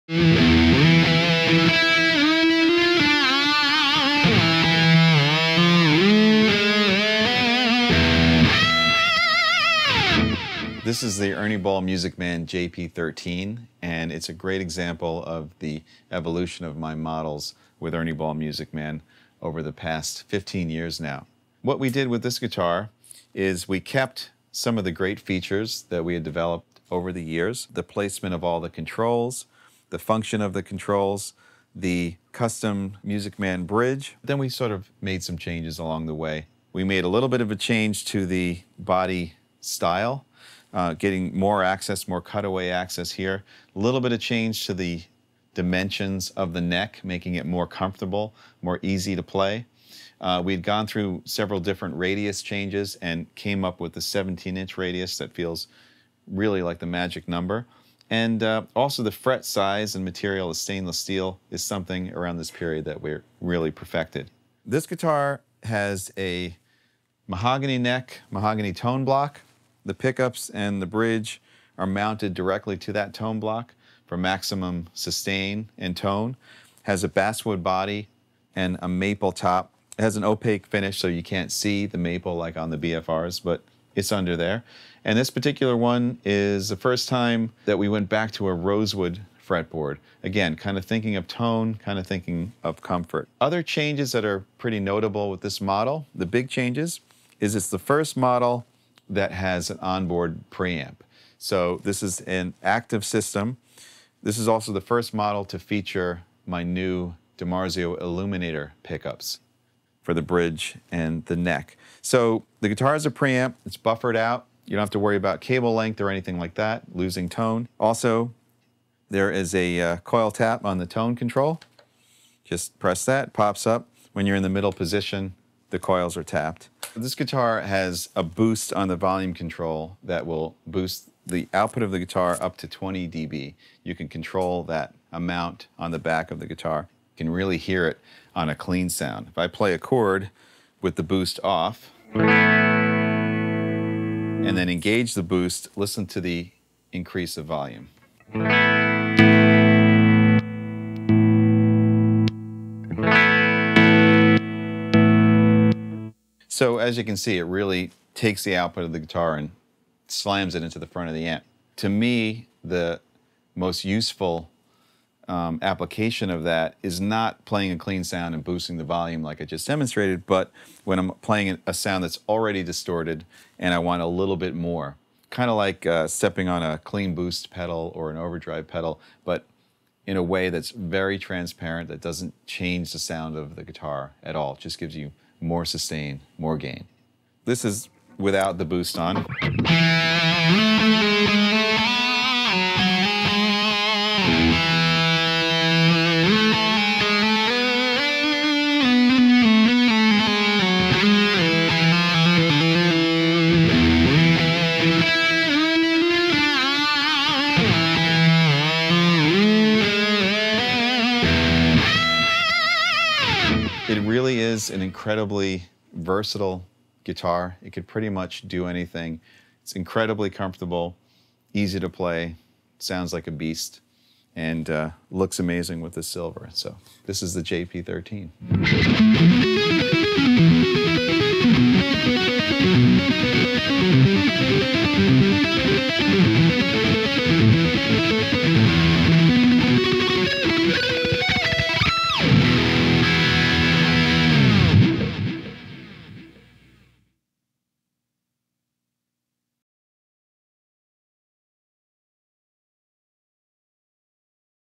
John Petrucci demos his Ernie Ball Music Man JP13
Guitar virtuoso John Petrucci walks you through the JP13 John Petrucci electric guitar from Ernie Ball Music Man.

Model:JP13
Description:Through experimentation while on the stage and in the studio, revelations both sonically and as they pertain to design have allowed me and the gifted team at MM to update and improve the features of the JP over the years. The 13 marks the first time a preamp circuit has been introduced in this guitar along with a solo boost conveniently accessed from the volume knob. The enhanced tonal range and openness is noticeable and welcomed from the moment you plug it in. - John Petrucci
Size:12.375" wide, 1.75" thick, 37" long (31.4cm wide, 4.5cm thick, 94cm long)
Weight:7 lbs, 4oz (3.29 kg) - varies slightly
Body Wood:Basswood with maple top and mahogany Tone Block
Bridge:Custom John Petrucci Music Man® Piezo floating tremolo, made of chrome plated, hardened steel with solid steel saddles
Scale Length:25.5" (64.8cm)
Neck Radius:17" (43.2cm)
Headstock Size:Only 5.875" (14.9cm) long
Frets:24 - Medium jumbo profile, stainless steel
Neck Width:1.6875" (43mm) at nut, 2.25" (57.2mm) at last fret
Neck Wood:Select mahogany
Fingerboard:Rosewood
Fret Markers:Custom JP13 Inlays
Neck Finish:High gloss polyester
Neck Colors:Color matches body
Tuning Machines:Schaller M6-IND locking with pearl buttons
Truss Rod:Adjustable - no component or string removal
Neck Attachment:5 bolts - perfect alignment with no shifting; sculpted neck joint allows smooth access to higher frets
Controls:Custom Music Man® active preamp; push/push volume for gain boost, 500kΩ push/push passive tone for custom 2 pickup configurations − .022µF tone capacitor
Switching:3-way toggle pickup selector, with custom center position configuration; 3-way toggle piezo/magnetic selector
Pickups:HH - DiMarzio Illuminator Bridge and Neck Humbuckers Piezo bridge pickup
Left Handed:No
Strings:6 String: 10p-13p-17p-26-36-46 (RPS 10 Slinkys #2240) 7 String: 10p-13p-17p-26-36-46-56 (RPS 10 Slinkys #2240 with added P01156)